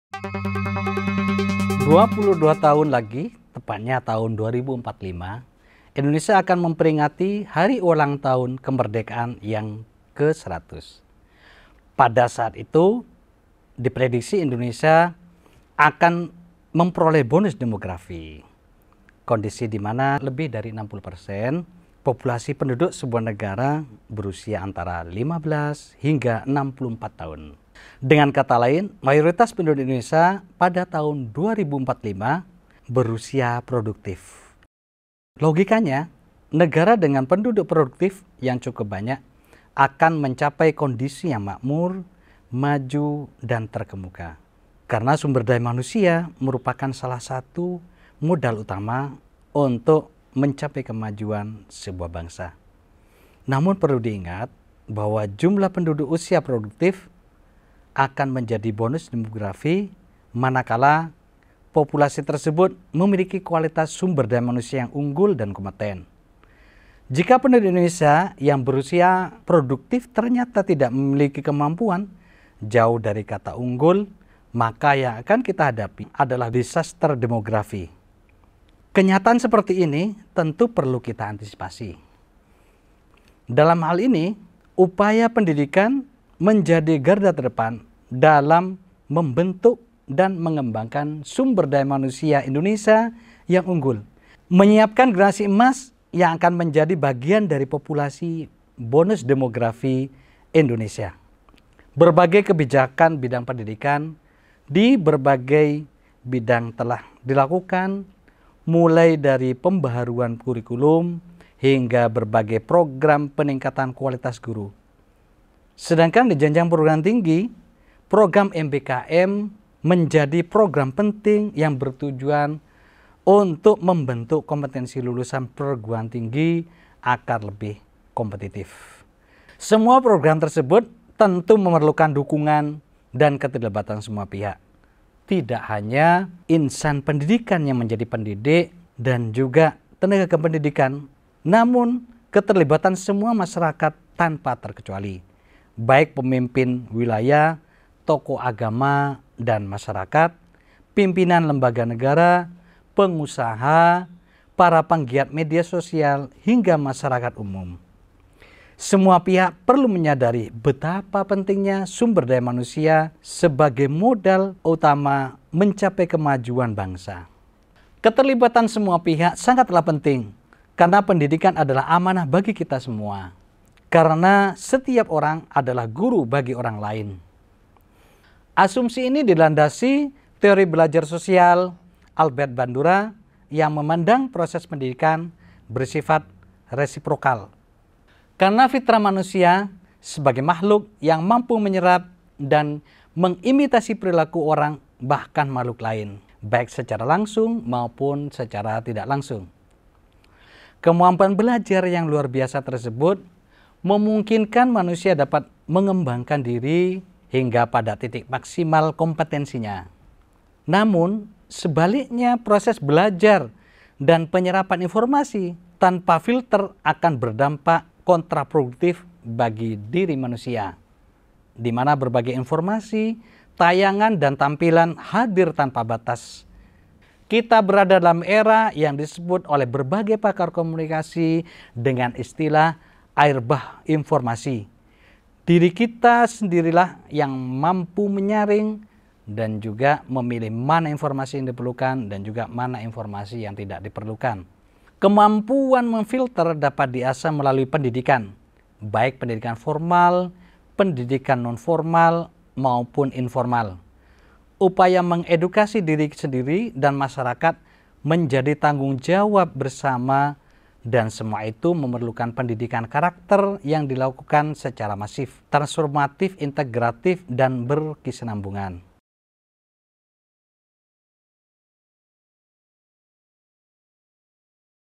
UM Bicara | Pendidikan Karakter - Dr. Ahmad Yusuf Sobri, S.Sos, M.Pd
Tahukah kamu bahwa bencana demografi salah satunya disebabkan oleh buruknya sistem pendidikan? Pada video ini, Dr. Ahmad Yusuf Sobri akan menjelaskan pentingnya pendidikan karakter untuk menyiapkan bonus demografi bagi Indonesia di tahun 2045.